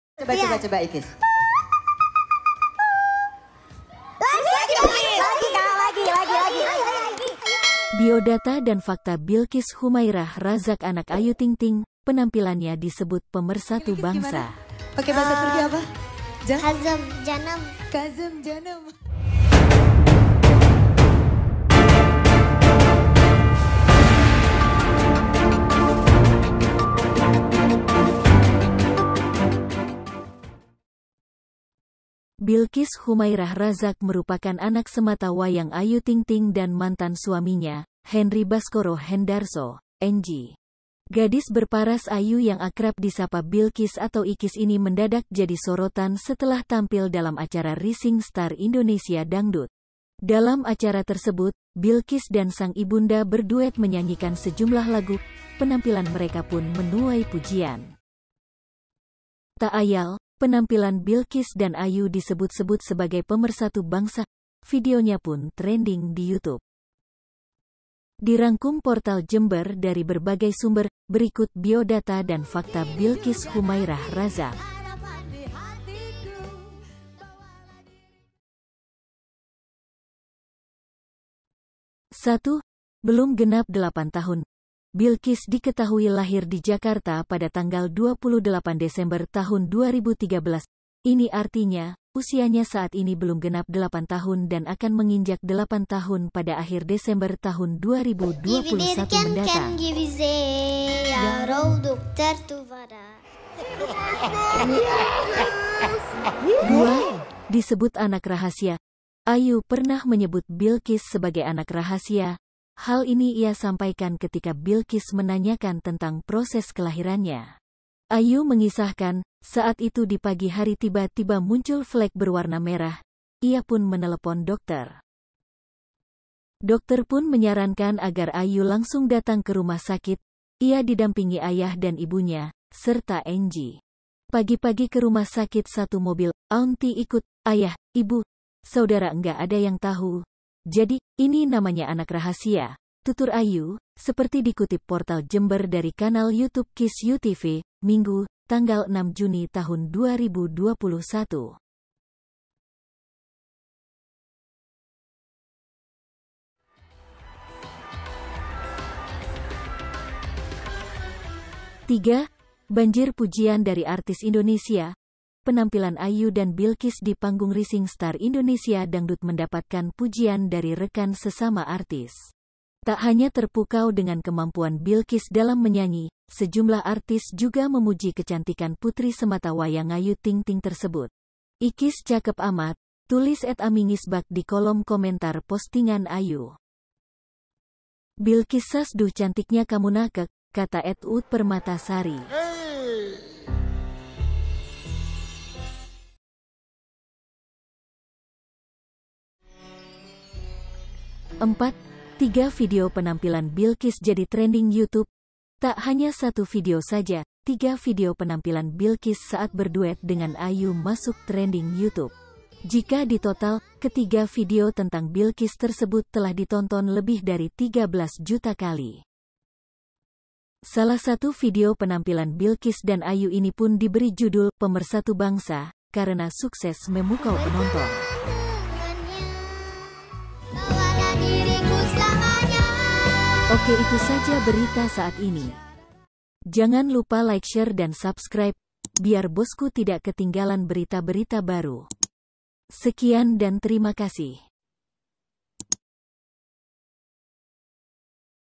KEREN BANGET BILKIS ‼️Bersua EMAS Bikin Heboh Se-Indonesia, Cek Bionata Bilkis
Biodata dan Fakta Bilqis Khumairah Razak Anak Ayu Ting Ting, Penampilannya Disebut Pemersatu Bangsa.
Bilqis Khumairah Razak merupakan anak semata wayang Ayu Ting Ting dan mantan suaminya, Henry Baskoro Hendarso (Enji)
Gadis berparas ayu yang akrab disapa Bilqis atau Iqis ini mendadak jadi sorotan setelah tampil dalam acara Rising Star Indonesia Dangdut.
Dalam acara tersebut, Bilqis dan sang ibunda berduet menyanyikan sejumlah lagu. Penampilan mereka pun menuai pujian.
Tak ayal, penampilan Bilqis dan Ayu disebut-sebut sebagai pemersatu bangsa. Videonya pun trending di Youtube.
Dirangkum Portal Jember dari berbagai sumber, berikut biodata dan fakta Bilqis Khumairah Razak.

🇮🇩🇲🇾 Hai Sobat Aku Semuanya Indonesia Dan Malaysia, Apa Kabar hari ini!

Jangan lewatkan video ini ya bos Qu